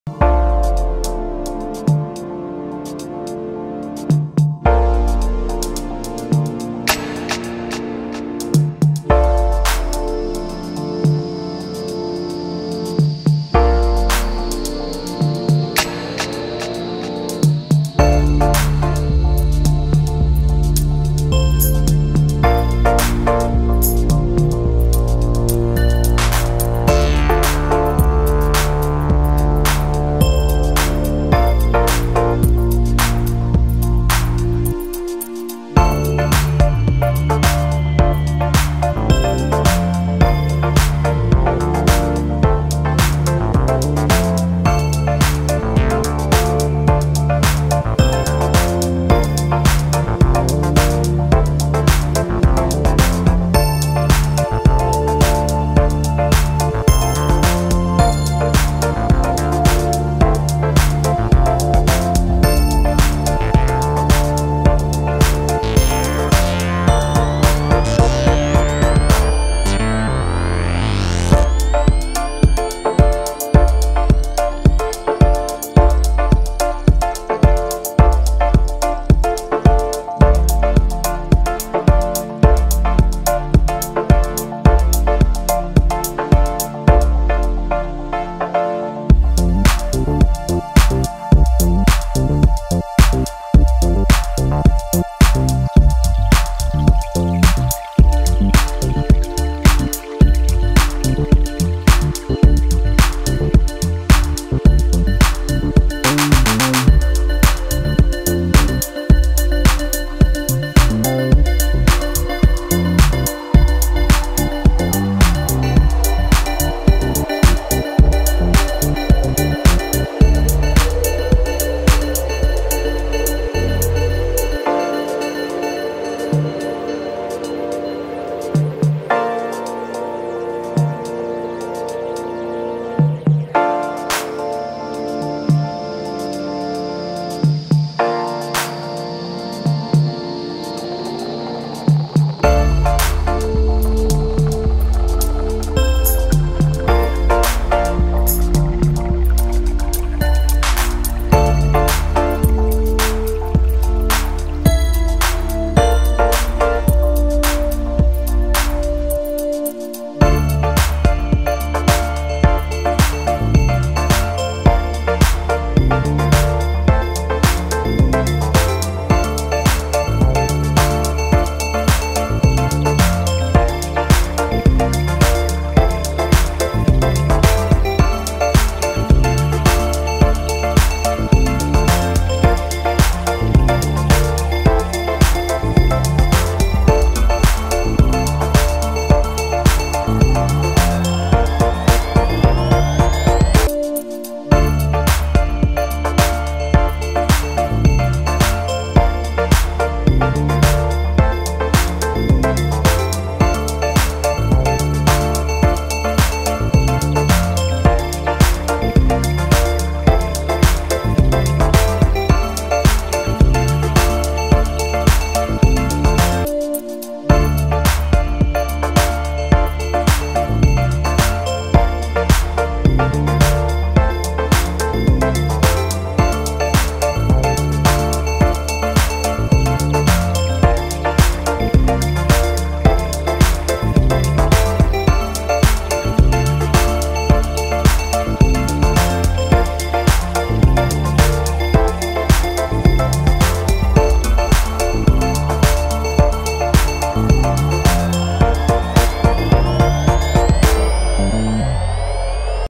limon Çekirdeği Çimlendirme
Limon çekirdeği nasıl çimlendirilir? Limon ağacı nasıl oluşturulur?videomuzu izleyerek uygulamalı bir şekilde öğrenebilirsiniz.

HAMZA METE VE ANNESİ
İYİ SEYİRLER...